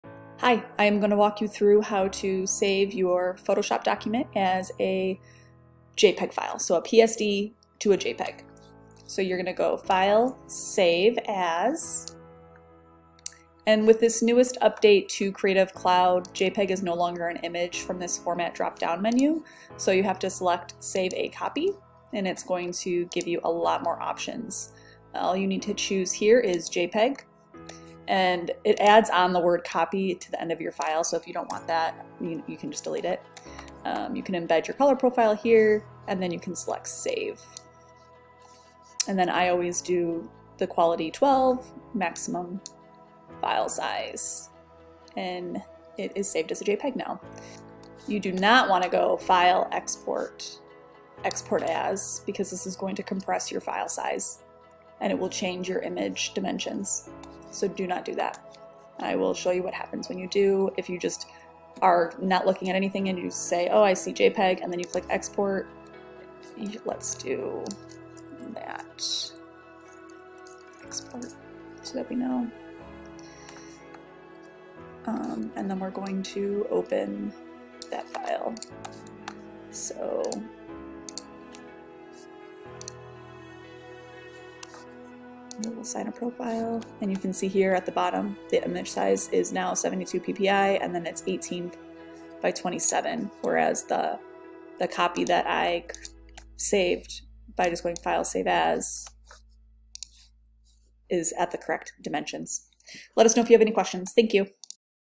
IC Quick Tip | How to save a JPG file
Learn how to save a JPG file from an original PSD file in the current update for Adobe Creative Cloud.